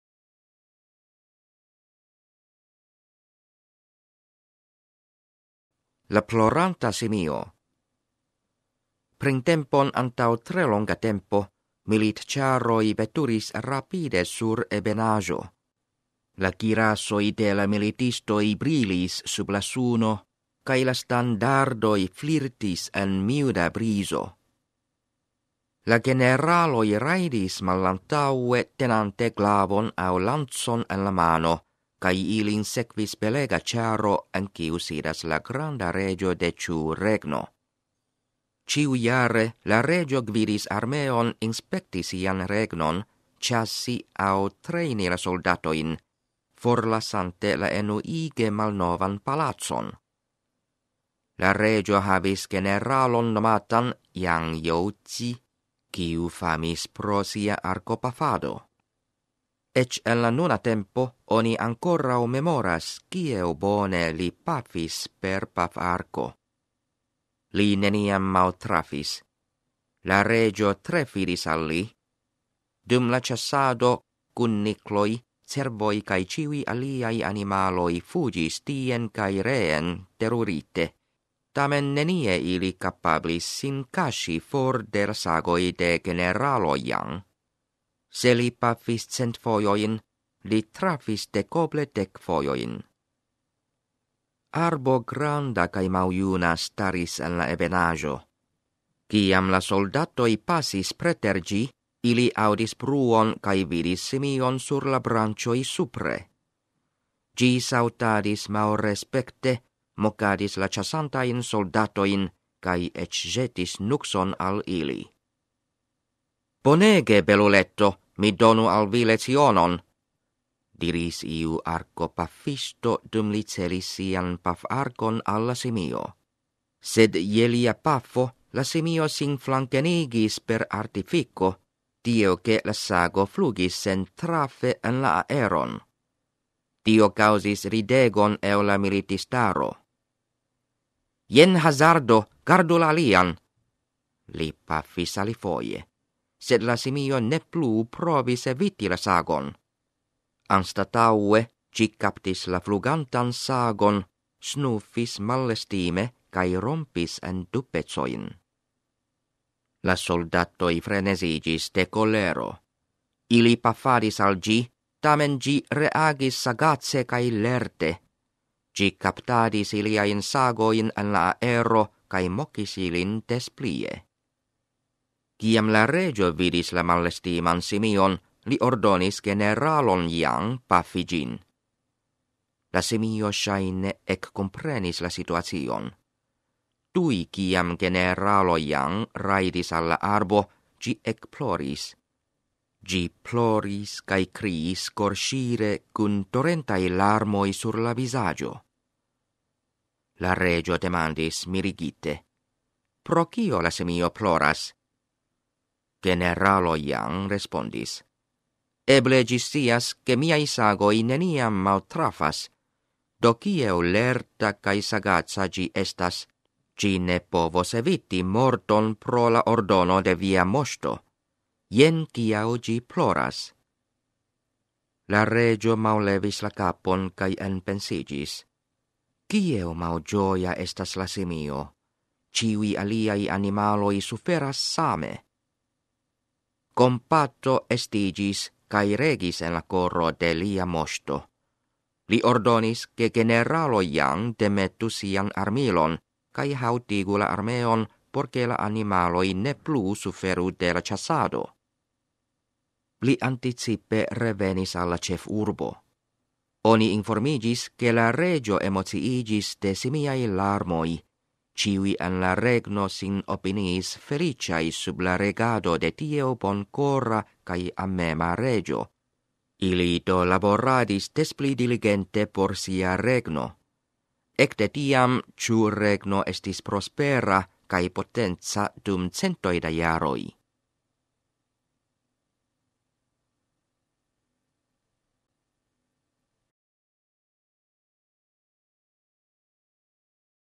1.La Ploranta Simio
Rakonto el AMO AL LA VIVOJ esperantigita de Miaohui kaj voĉlegita de s-ro Zhao Jianping el CRI. Por pliaj legaĵoj bonvolu viziti Budhisma Retejo Esperantista